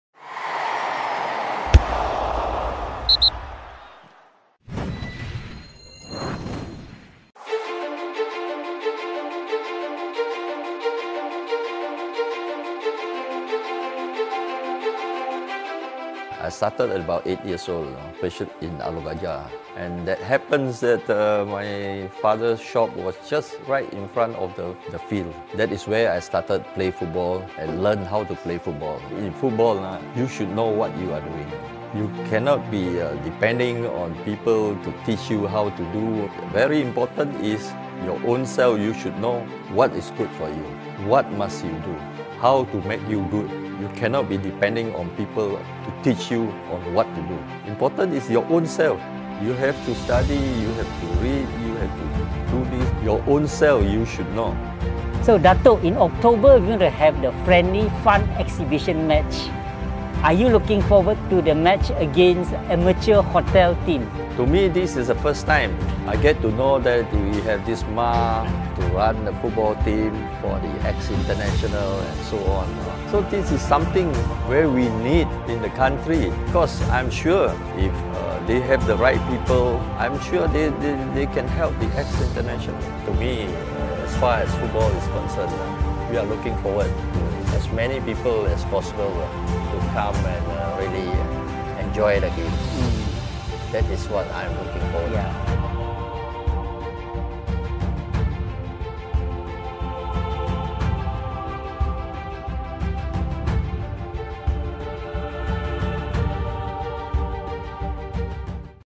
MAH Football Tournament Pre Game
The inaugural MAH Football Tournament Pre-Game Video Featuring the Legend, Datuk Wira Soh Chin Ann.